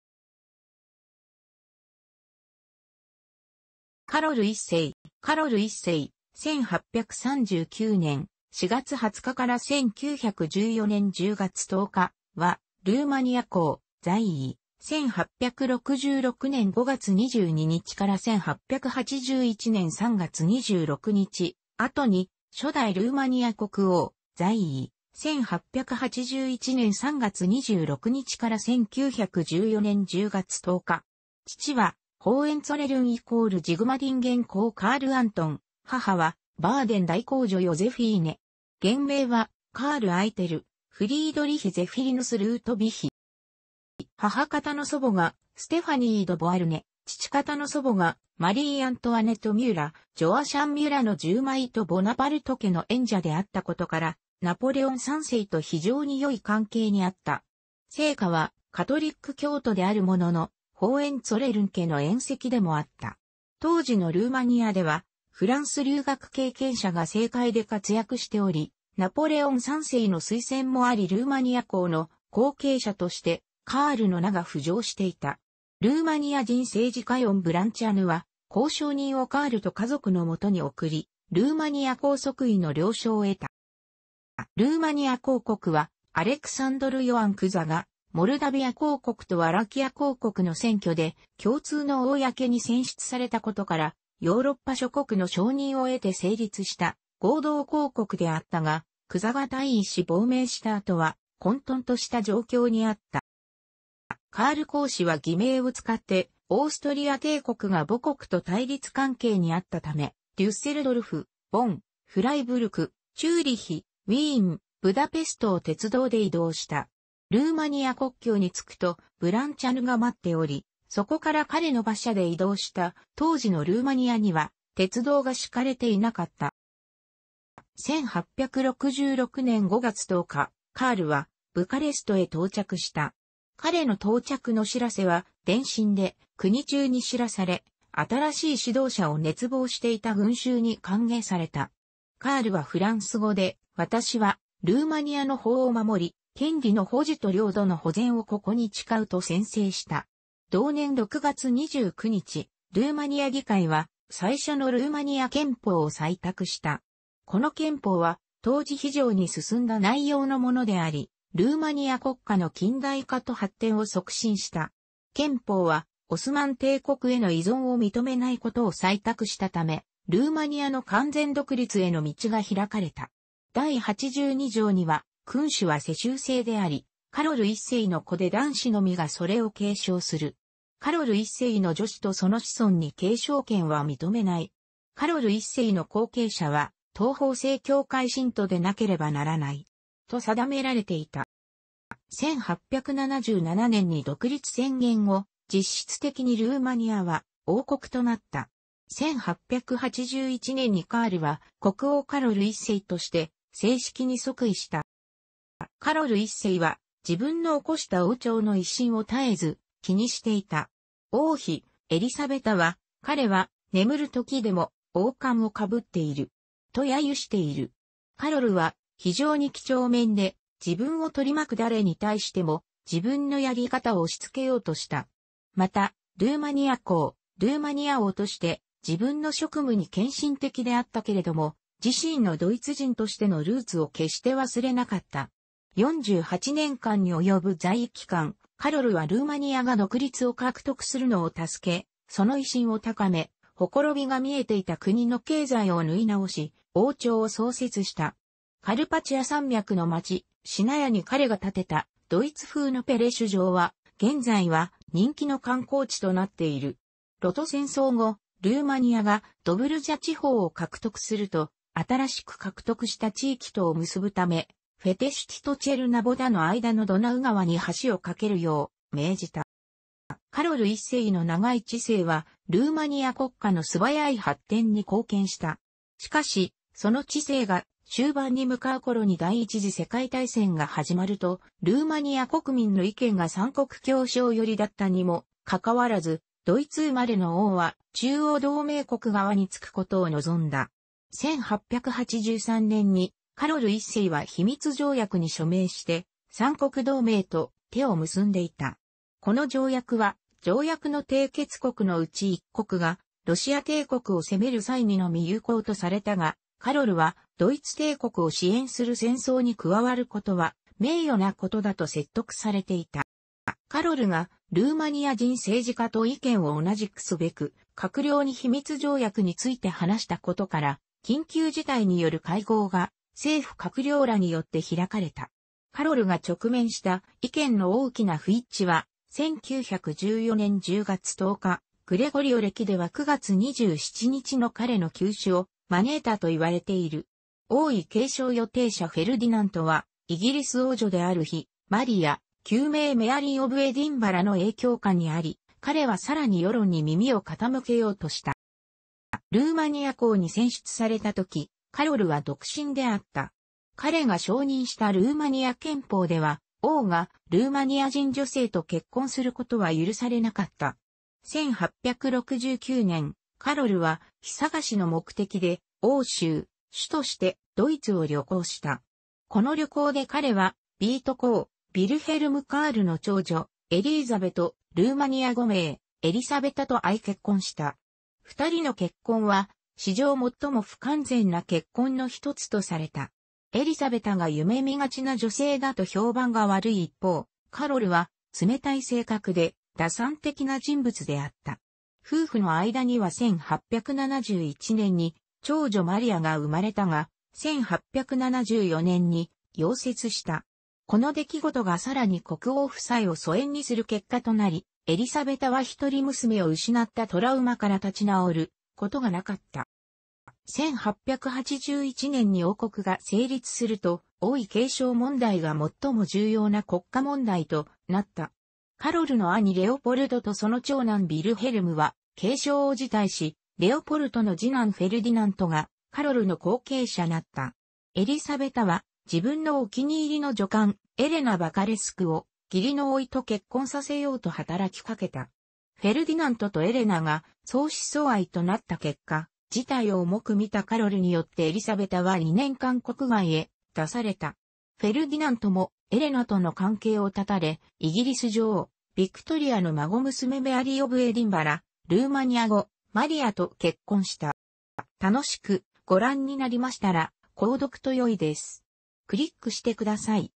カロル1世 (ルーマニア王)
カロル1世（、1839年4月20日 - 1914年10月10日）は、ルーマニア公（在位：1866年5月22日 - 1881年3月26日）、後に初代ルーマニア国王（在位：1881年3月26日 - 1914年10月10日）。父はホーエンツォレルン＝ジグマリンゲン侯カール・アントン、母はバーデン大公女ヨゼフィーネ。原名はカール・アイテル・フリードリヒ・ゼフィリヌス・ルートヴィヒ（）。

母方の祖母がステファニー・ド・ボアルネ、父方の祖母がマリー・アントワネット・ミュラ（ジョアシャン・ミュラの従妹）とボナパルト家の縁者であったことから、ナポレオン3世と非常に良い関係にあった。生家はカトリック教徒であるものの、ホーエンツォレルン家の縁戚でもあった。当時のルーマニアではフランス留学経験者が政界で活躍しており、ナポレオン3世の推薦もありルーマニア公の後継者としてカールの名が浮上していた。ルーマニア人政治家イオン・ブランチャヌは交渉人をカールと家族の元に送り、ルーマニア公即位の了承を得た。

ルーマニア公国は、アレクサンドル・ヨアン・クザがモルダヴィア公国とワラキア公国の選挙で共通の公に選出されたことから、ヨーロッパ諸国の承認を得て成立した合同公国であったが、クザが退位し亡命した後は混沌とした状況にあった。

カール侯子は偽名を使って（オーストリア帝国が母国と対立関係にあったため）、デュッセルドルフ、ボン、フライブルク、チューリッヒ、ウィーン、ブダペストを鉄道で移動した。ルーマニア国境に着くとブランチャヌが待っており、そこから彼の馬車で移動した（当時のルーマニアには鉄道が敷かれていなかった）。

1866年5月10日、カールはブカレストへ到着した。彼の到着の知らせは電信で国中に知らされ、新しい指導者を熱望していた群衆に歓迎された。カールはフランス語で『私はルーマニアの法を守り、権利の保持と領土の保全をここに誓う』と宣誓した。同年6月29日、ルーマニア議会は最初のルーマニア憲法を採択した。この憲法は当時非常に進んだ内容のものであり、ルーマニア国家の近代化と発展を促進した。憲法は、オスマン帝国への依存を認めないことを採択したため、ルーマニアの完全独立への道が開かれた。第82条には『君主は世襲制であり、カロル1世の子で男子のみがそれを継承する。カロル1世の女子とその子孫に継承権は認めない。カロル1世の後継者は東方正教会信徒でなければならない。』と定められていた。

1877年に独立宣言後、実質的にルーマニアは王国となった。1881年にカールは国王カロル1世として正式に即位した。

カロル1世は自分の興した王朝の威信を絶えず気にしていた。王妃エリサベタは、『彼は眠るときでも王冠を被っている。』と揶揄している。カロルは非常に几帳面で、自分を取り巻く誰に対しても自分のやり方を押しつけようとした。また、ルーマニア公、ルーマニア王として自分の職務に献身的であったけれども、自身のドイツ人としてのルーツを決して忘れなかった。48年間に及ぶ在位期間、カロルはルーマニアが独立を獲得するのを助け、その威信を高め、ほころびが見えていた国の経済を縫い直し、王朝を創設した。カルパチア山脈の町シナヤに彼が建てたドイツ風のペレシュ城は、現在は人気の観光地となっている。露土戦争後、ルーマニアがドブルジャ地方を獲得すると、新しく獲得した地域とを結ぶためフェテシュティとチェルナヴォダの間のドナウ川に橋を架けるよう命じた。

カロル1世の長い治世は、ルーマニア国家の素早い発展に貢献した。しかし、その治世が終盤に向かう頃に第一次世界大戦が始まると、ルーマニア国民の意見が三国協商寄りだったにもかかわらず、ドイツ生まれの王は中央同盟国側に就くことを望んだ。1883年にカロル1世は秘密条約に署名して、三国同盟と手を結んでいた。この条約は、条約の締結国のうち一国がロシア帝国を攻める際にのみ有効とされたが、カロルはドイツ帝国を支援する戦争に加わることは名誉なことだと説得されていた。

カロルがルーマニア人政治家と意見を同じくすべく、閣僚に秘密条約について話したことから、緊急事態による会合が政府閣僚らによって開かれた。カロルが直面した意見の大きな不一致は、1914年10月10日（グレゴリオ暦では9月27日）の彼の急死を招いたといわ...